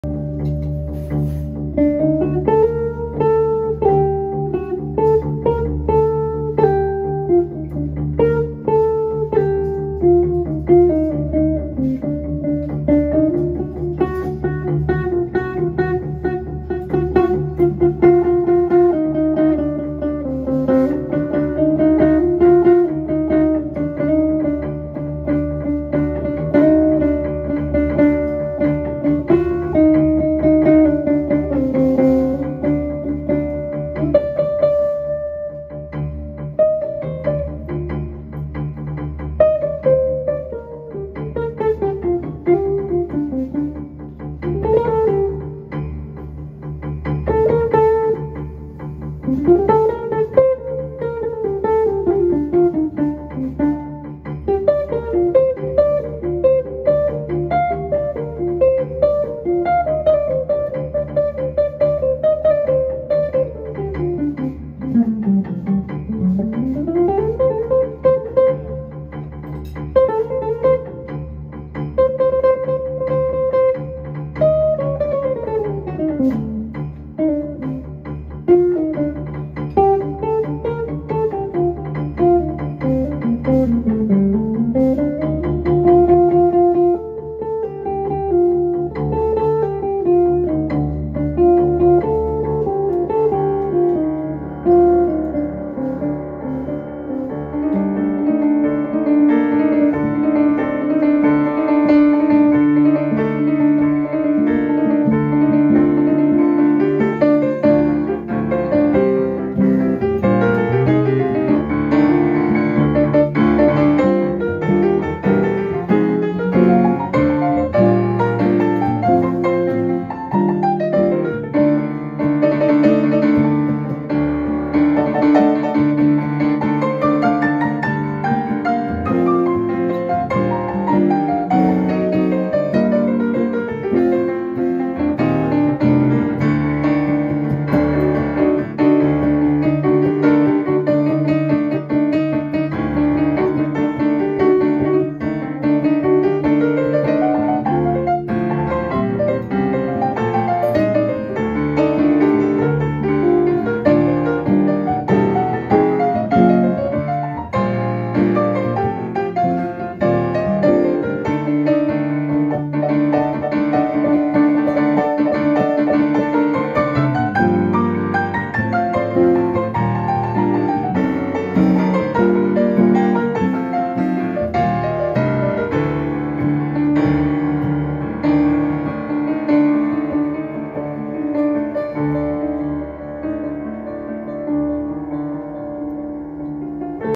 Abe Rábade & Rale Micic live at Blue Note
Abe Rábade and Rale Micic Duo
Live at @bluenotenewyork1442